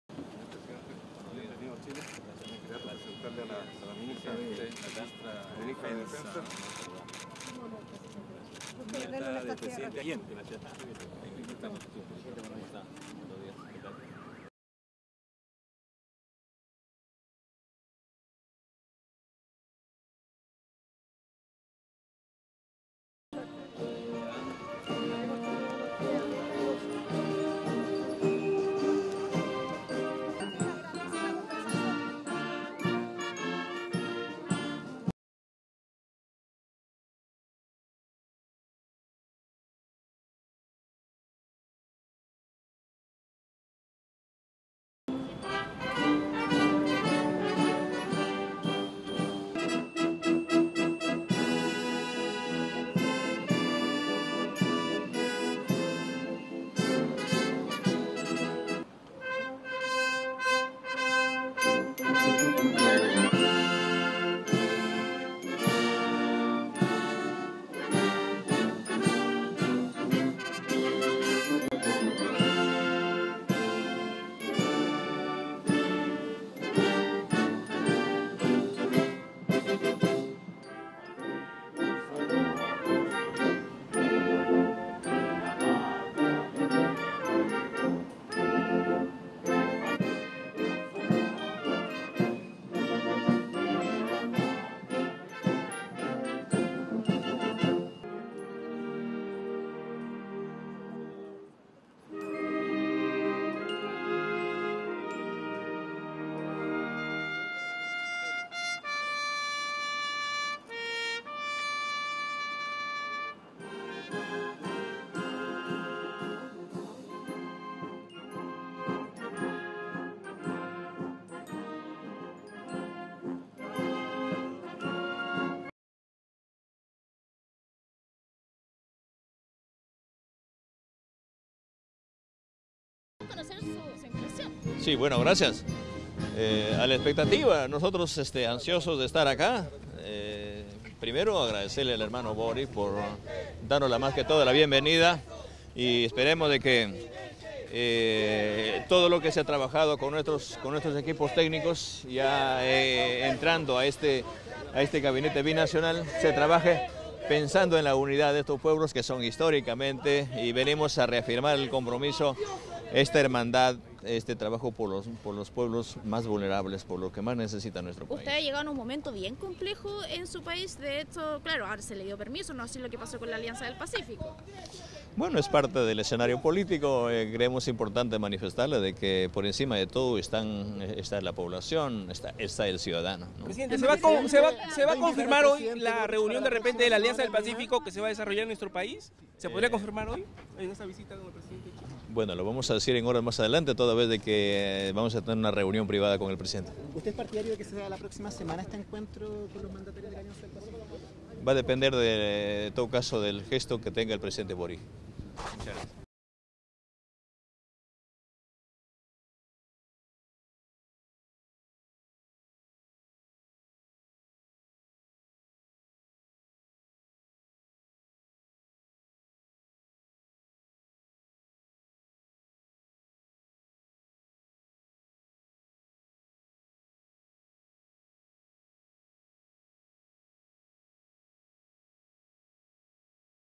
Chile | Ceremonia de colocación de ofrenda floral ante el monumento de Bernardo O ́Higgins
El presidente Pedro Castillo Terrones, junto con ministros de Estado, participa en la ceremonia de colocación de ofrenda floral ante el monumento del libertador Bernardo O ́Higgins, en Santiago de Chile.